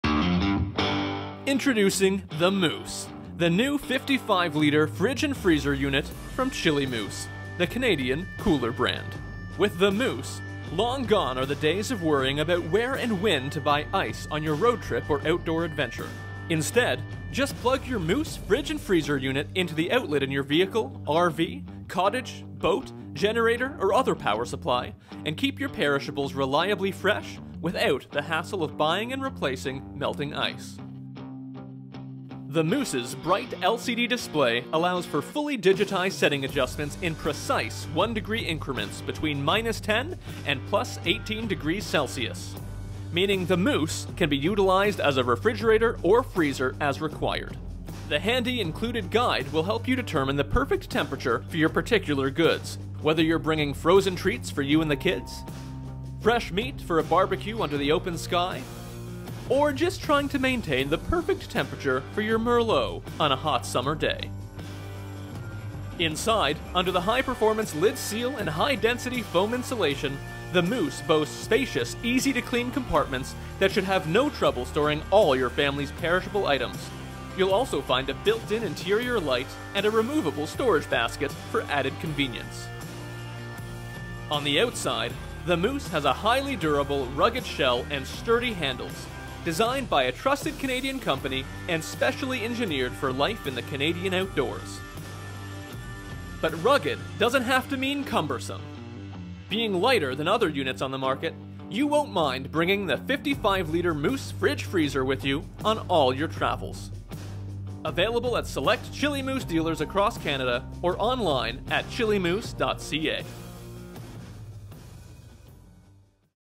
Chilly Moose - The Moose Portable Fridge-Freezer
55L Chilly Moose Portable Fridge-Freezer

The new 55L Moose portable 12V refrigerated cooler is the ultimate powered cooler for outdoor living. Keep your food reliably fresh and cold for as long as you need it, without the hassle of melting ice.